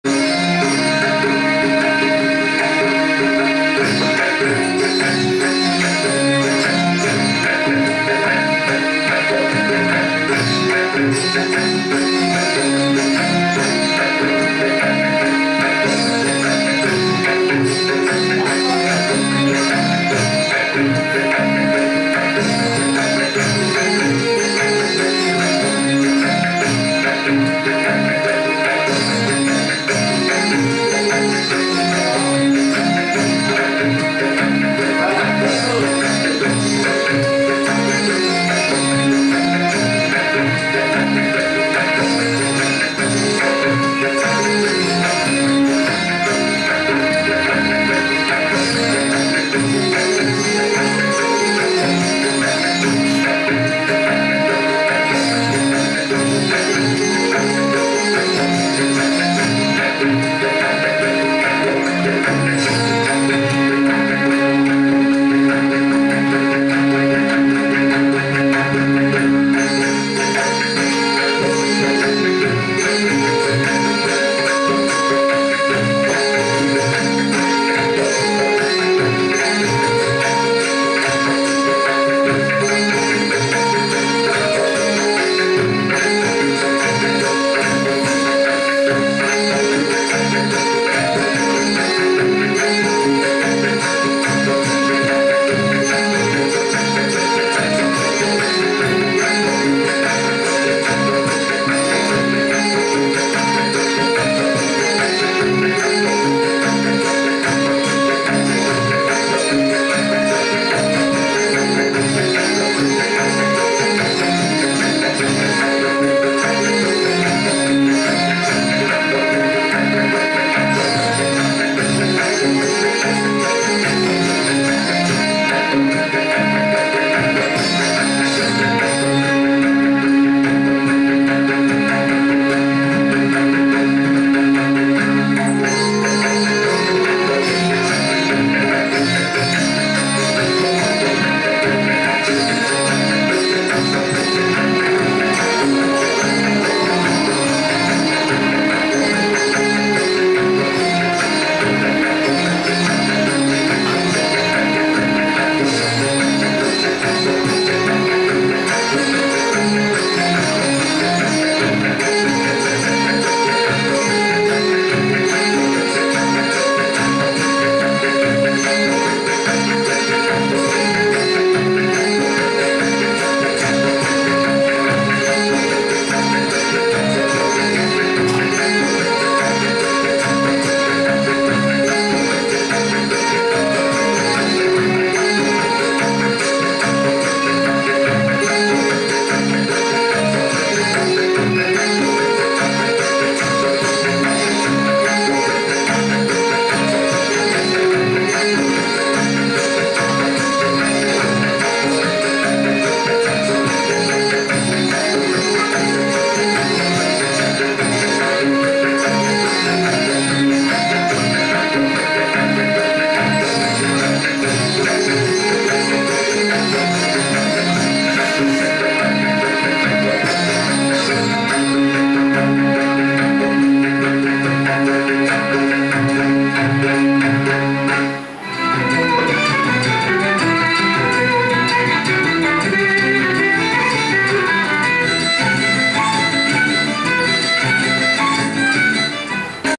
MODERN KIDS TV
Sendhoori's keyboard concert on Sankara TV on Diwali 2019